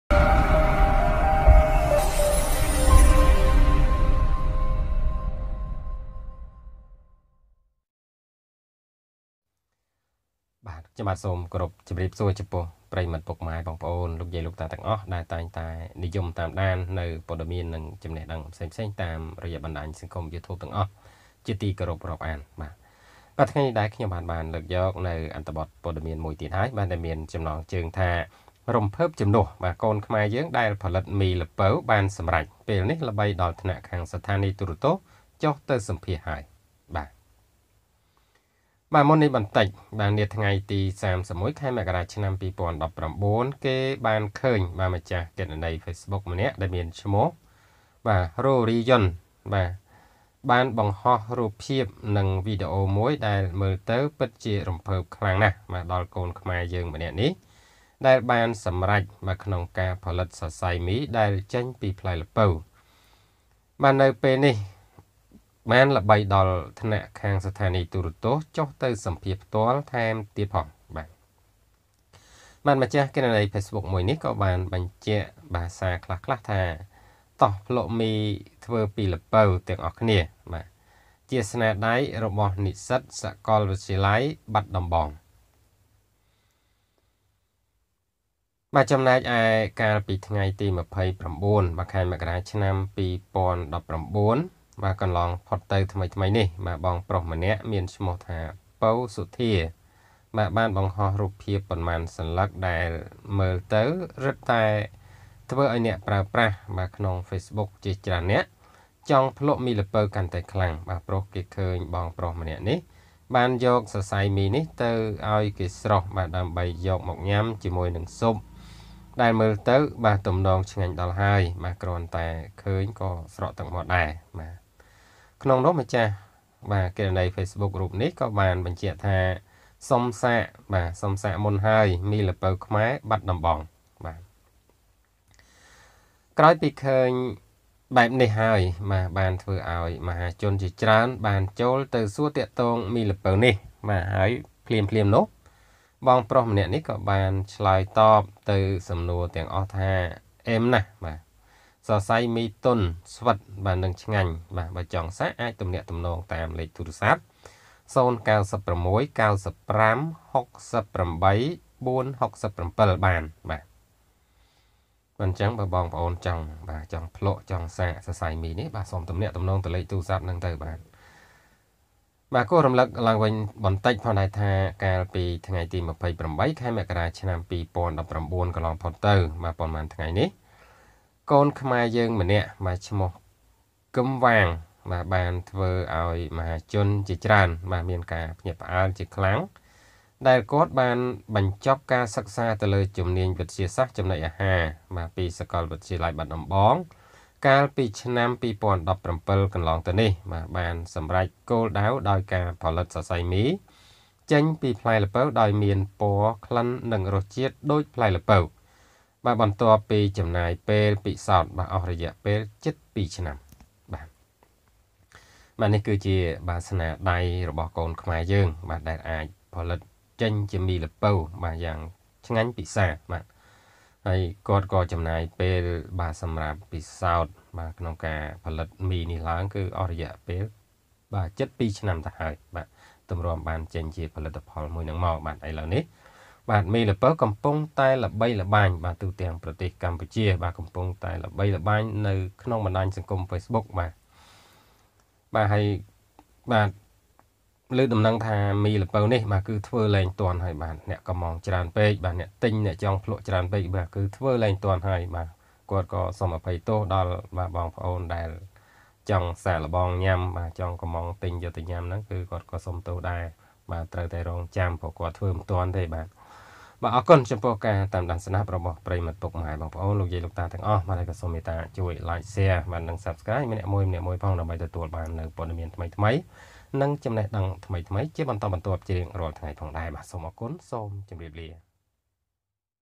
រំភើបជំនួស! កូនខ្មែរយើងដែលផលិត “មីល្ពៅ” បានសម្រេច ពេលនេះល្បីដល់ថ្នាក់ខាងស្ថាននីយ៍ទូរទស្សន៍ចុះទៅសម្ភា
រំភើបជំនួស! កូនខ្មែរយើងដែលផលិត “មីល្ពៅ” បានសម្រេច ពេលនេះល្បីដល់ថ្នាក់ខាងស្ថាននីយ៍ទូរទស្សន៍ចុះទៅសម្ភាសន៍ហើយ